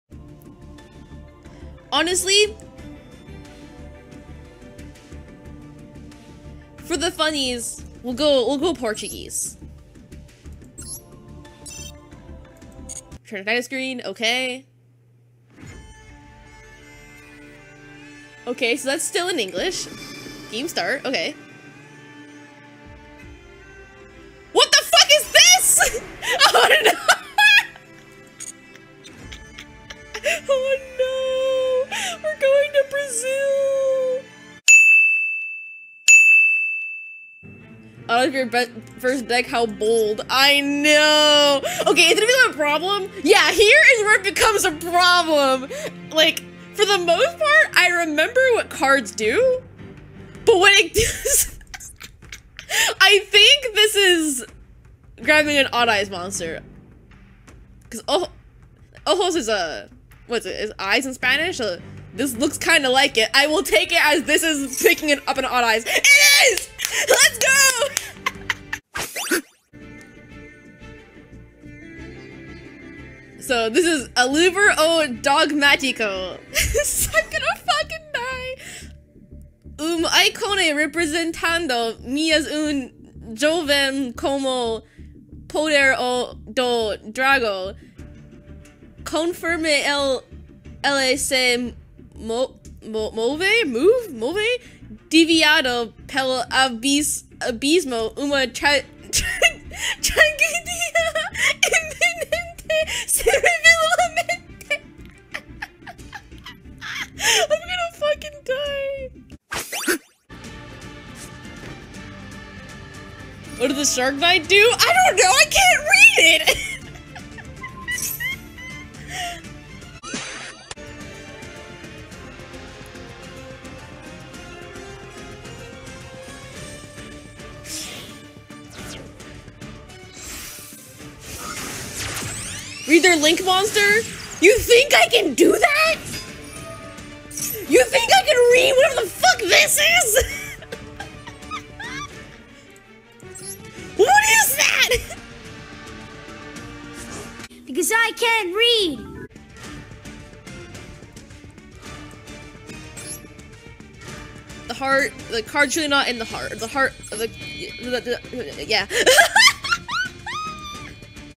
[VReverie Clips] Nova Aokami Can('t) Read Portuguese.
Original stream at 15:48, 20:10, 1:02:28, 1:23:20, 1:32:55 and 3:15:05

Oh Exodia, she's going to Brazil!